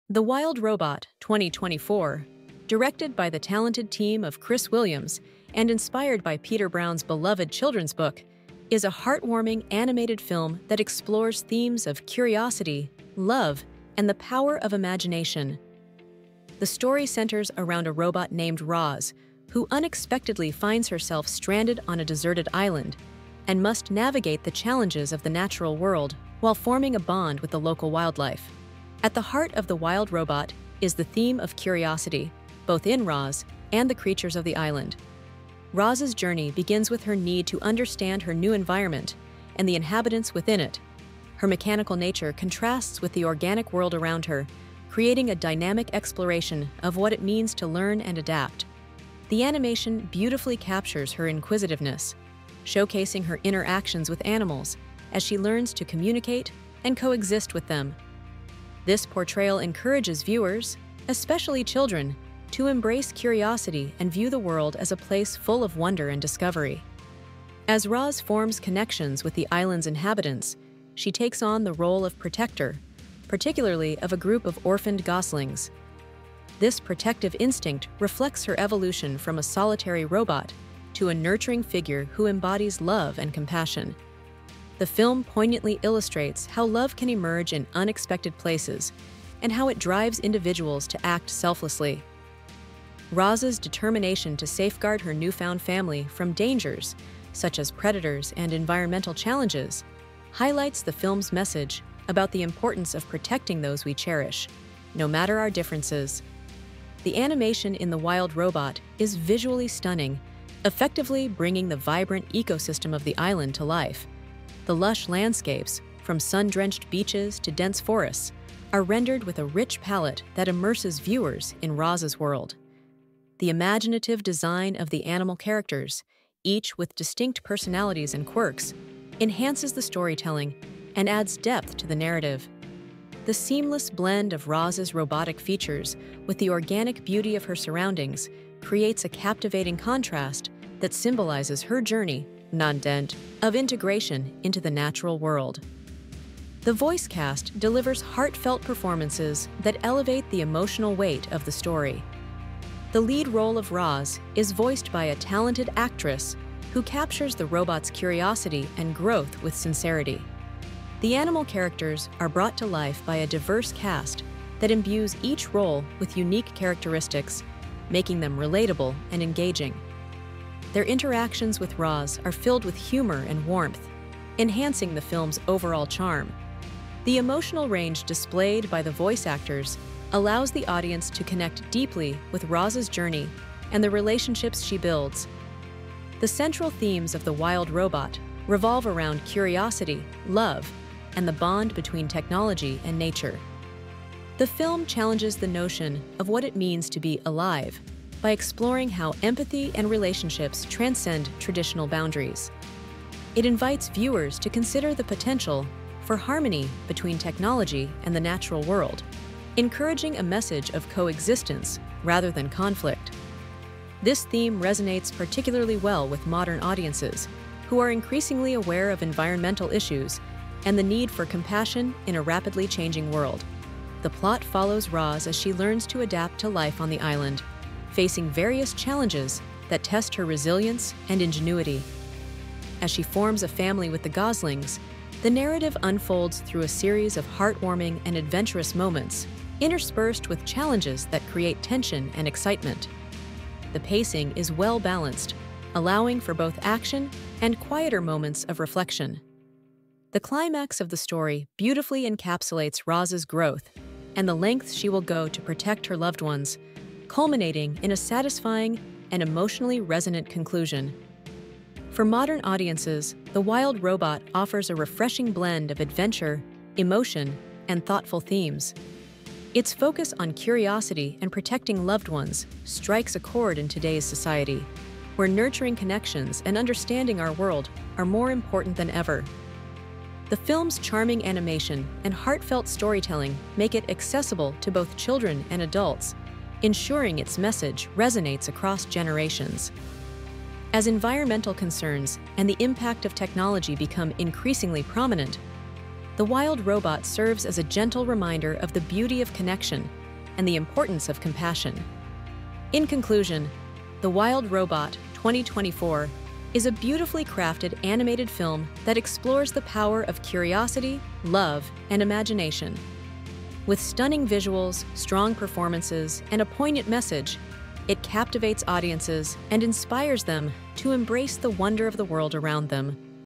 Film Review: The Wild Robot
The Wild Robot, written by Peter Brown, tells the heartfelt story of Roz, a robot who becomes stranded on a remote island after a shipwreck. Through the story, readers witness Roz’s journey as she adapts to the wilderness, learns to communicate with animals, and becomes a mother figure to a gosling named Brightbill. Despite being initially feared by the island’s animals, Roz wins their trust and builds strong bonds, redefining family and community. This engaging tale addresses themes of resilience, friendship, environmental stewardship, and the intersection of nature with technology, resonating deeply with both children and adults alike​